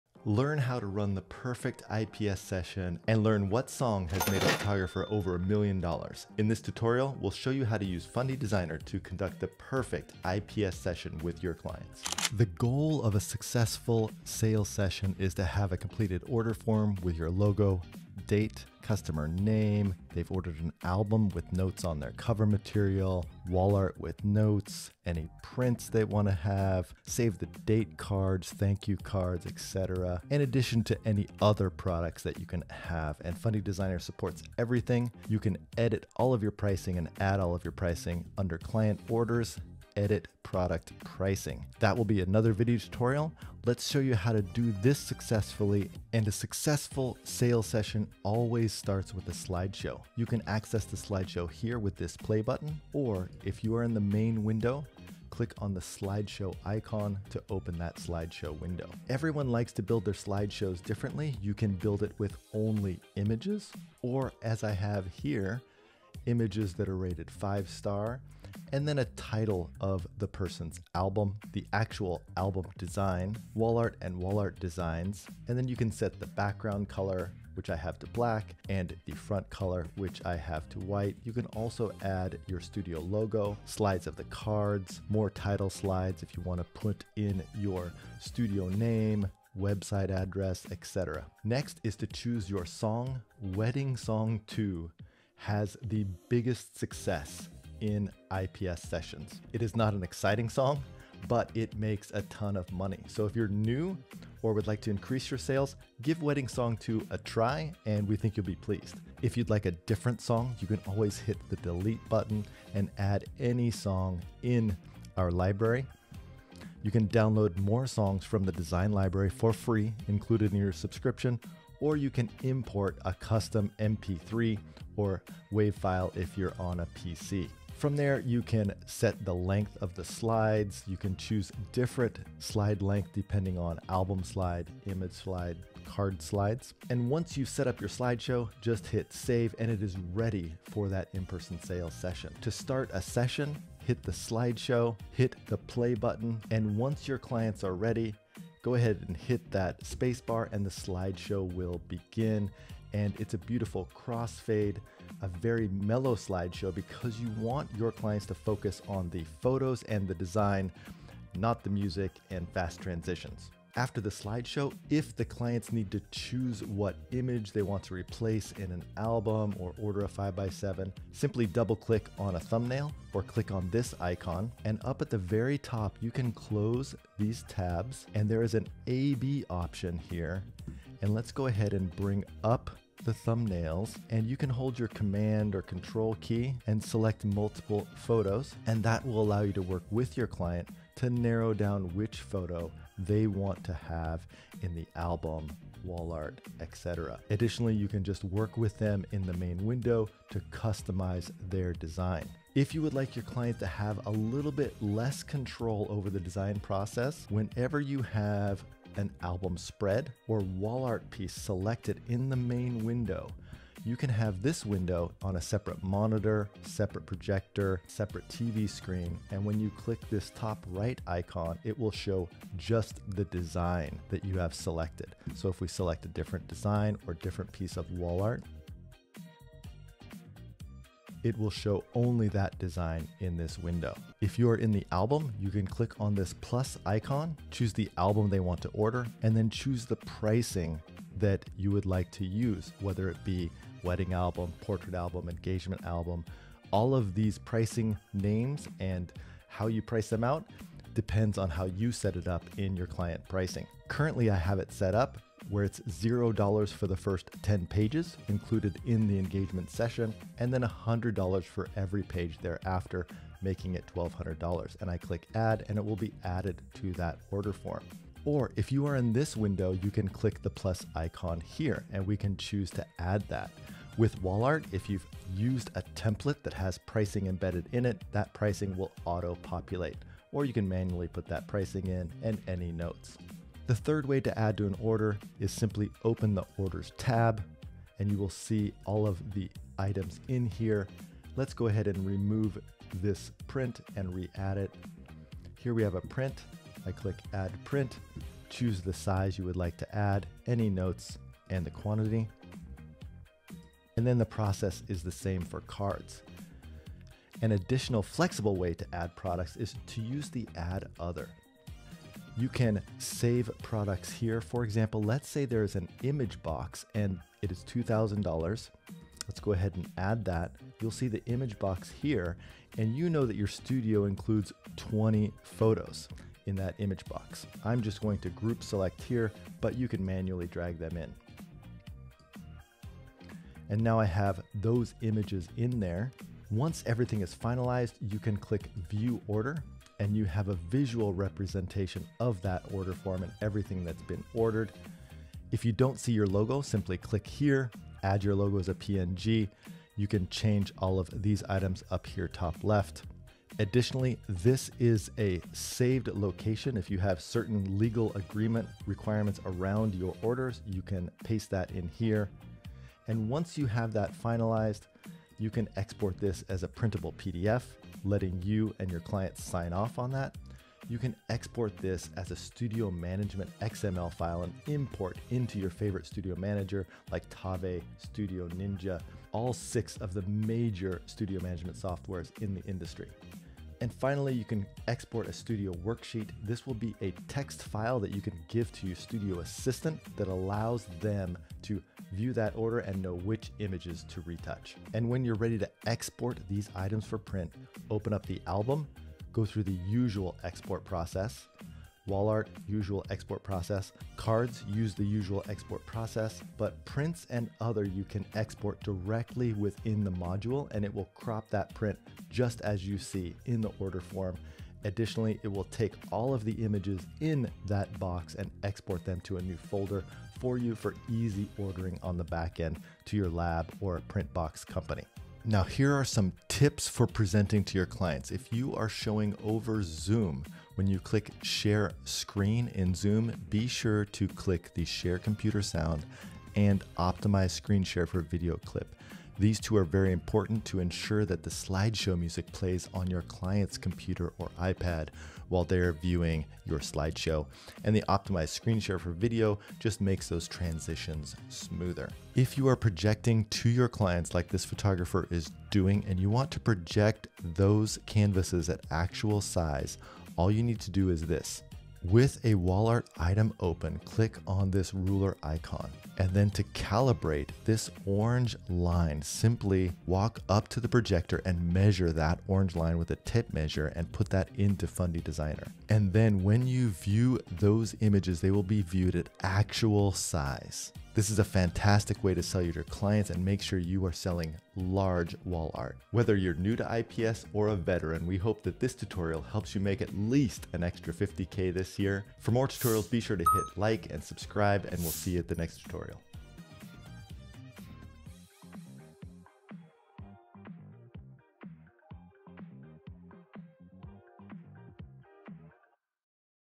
Mastering IPS: How to Conduct the Perfect Session with Fundy Designer
Learn how to run the perfect In-Person Sales (IPS) session and discover the song that has made a photographer over a million dollars! In this tutorial, we'll show you how to use Fundy Designer v11 to streamline your workflow and boost your sales.

Key Highlights:
➡️ Utilize the new Design Wizard for auto-designing albums, wall art, and cards.
➡️ Create distraction-free presentations with Theater Mode.
➡️ Customize and present your designs to clients seamlessly.
➡️ Leverage the power of slideshows to captivate your clients.
➡️ Efficiently manage orders, pricing, and client interactions.

Get ready to leave your clients speechless with stunning designs and efficient processes. Whether you're new to IPS or a seasoned pro, this tutorial is packed with tips to help you make an extra $50k this year.

Chapters:
0:00 Introduction and Overview
0:46 How to Create a Successful Slideshow
2:22 Presenting Slideshows
2:44  Selecting and Replacing Images During IPS
3:22 Theater Mode and Distraction-Free Presentations
3:54 Managing Orders and Pricing
5:36 How to View & Finalize Your Order Form
6:35 Exporting and Finalizing Orders
7:05 Tips for Presenting Over Zoom
7:32 Tips for Presenting In-Person
7:41 Calibrating Projector for Actual Size Viewing
8:06 Final Tips for Successful IPS Sessions